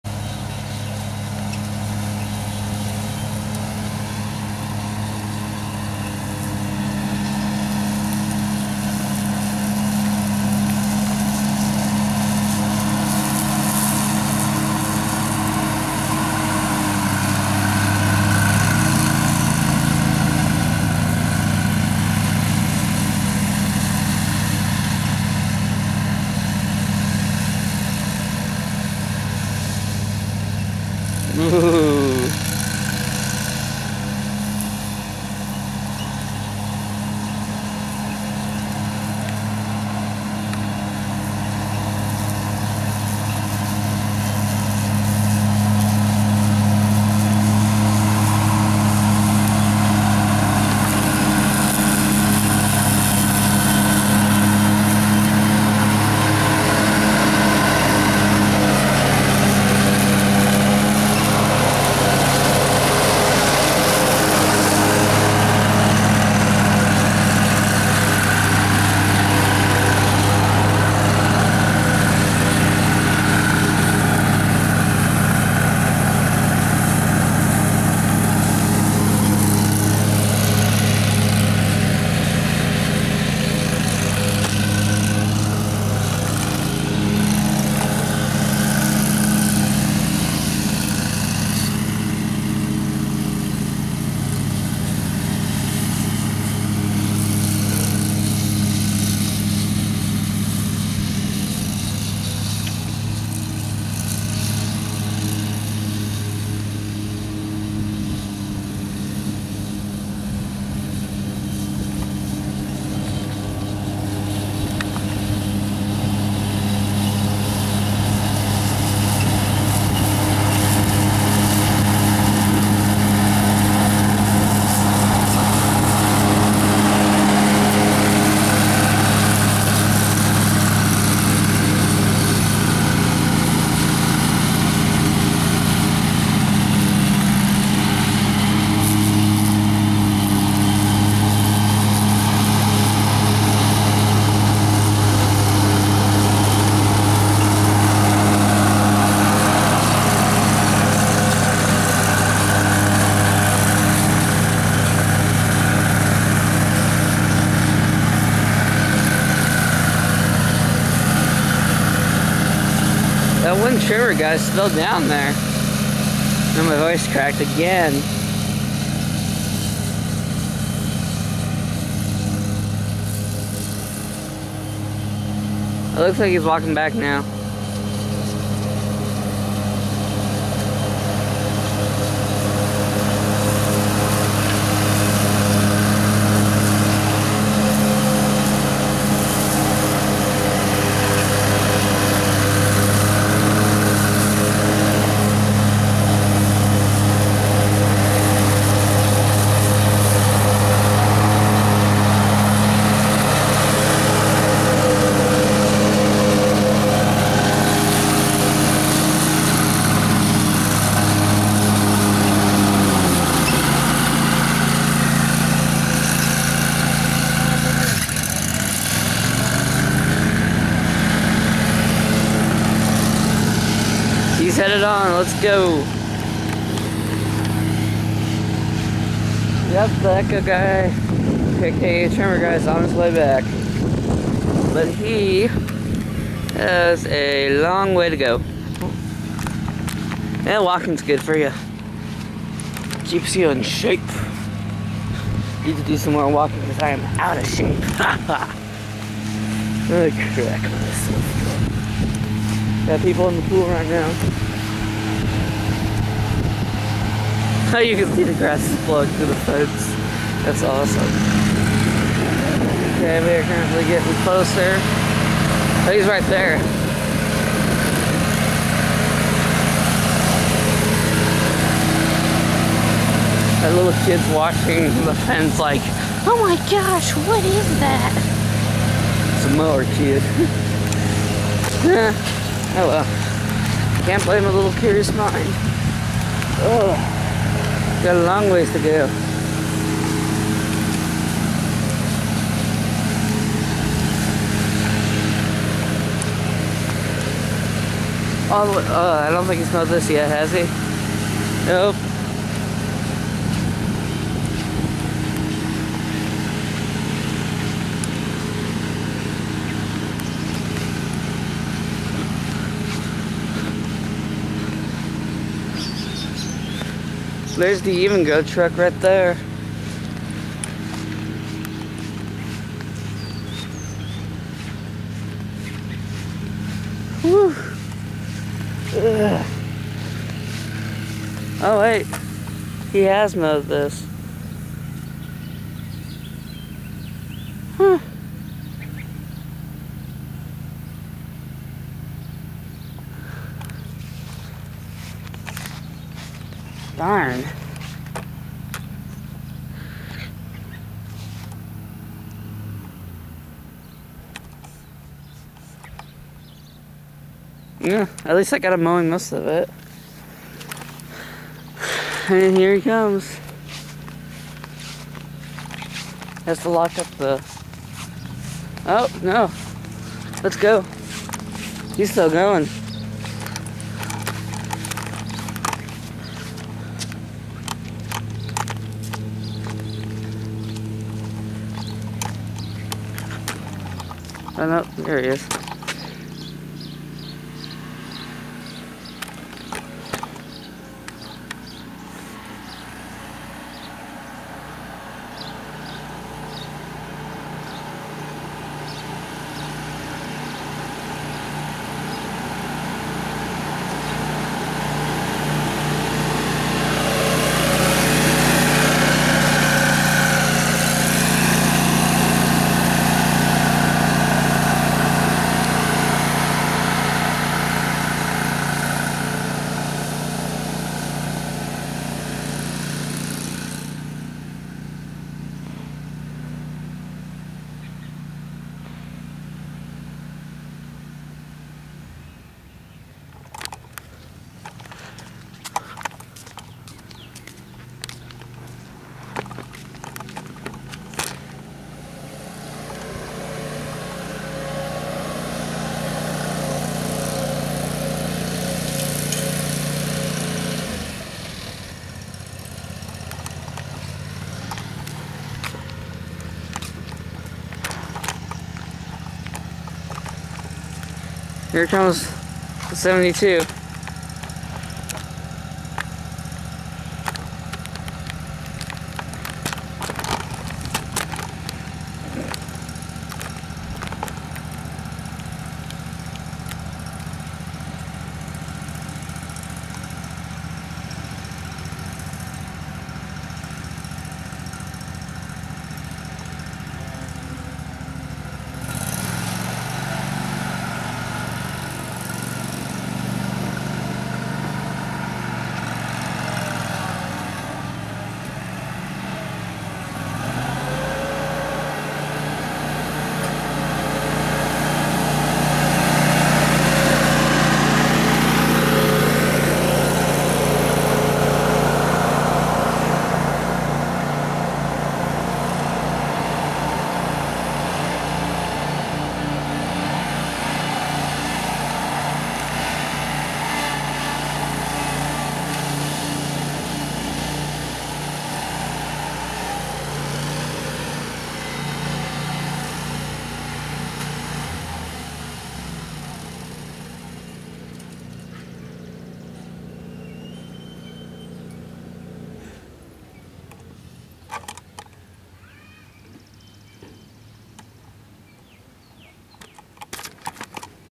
June Even Grow Action #2 (Part #10)
(Note: Do to video length, some videos uploaded faster than others so the order is messed up so sorry about that, just check the titles and you should set to go.
So they came on a Thursday and I wasn't expecting that so I wasn't ready and missed the scag walk-behind, so sorry. I did get a lot of toro action and some echo action. As the guys who trimming said, "I'm them famous!" These guys are awesome. Hopefully by making these videos, it will help there business.
Even Grow mowing action for the month of June will Feature:
#1 - Scag Turf Tiger 72" Mower (6/13/14) - Done
#2 - Toro 5000 Action6/20/14)
#3 - Scag Tiger Turf 60" Mower and Scag walk-behind (6/27/14)
In this video:
Scag Turf Tiger 72" mower
Toro Z Master 5000 series
Scag advantage 52
Echo SRM-266
Echo PB-770